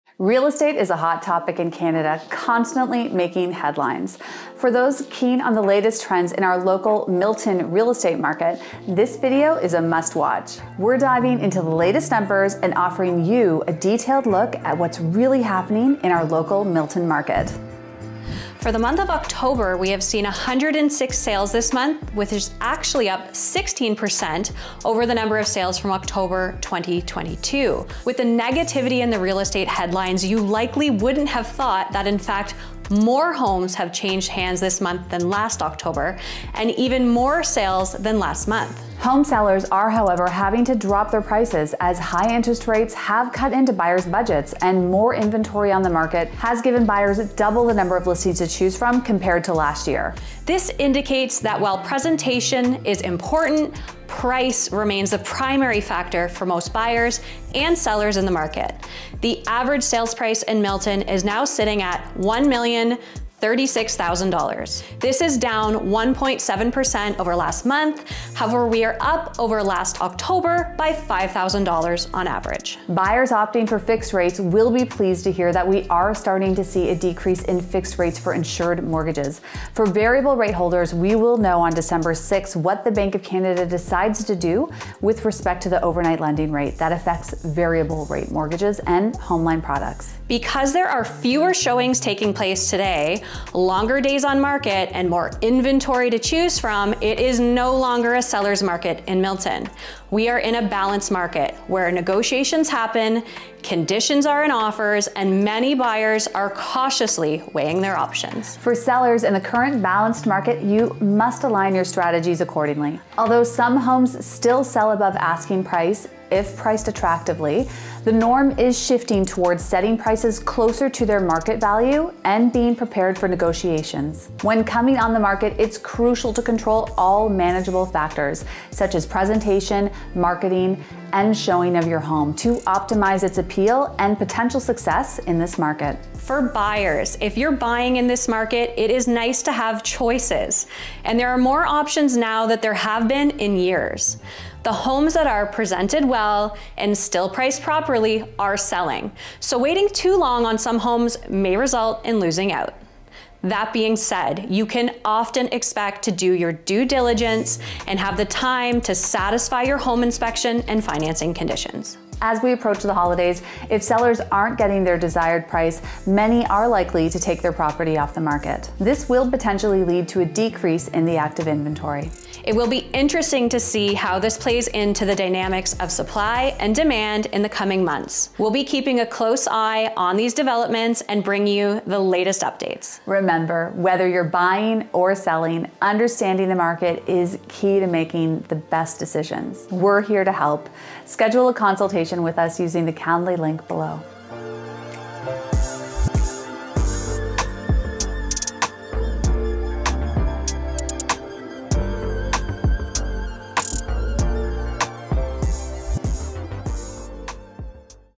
Milton Real Estate Market Update | October 2023
In this video, we will talk about the current real estate market in Milton and give you a glimpse into what we may expect in the future.

Whether you're buying or selling a home in Milton, don't miss this important update on the Milton real estate market! We'll cover the latest prices and trends and give you a snapshot of what you may expect in the near future.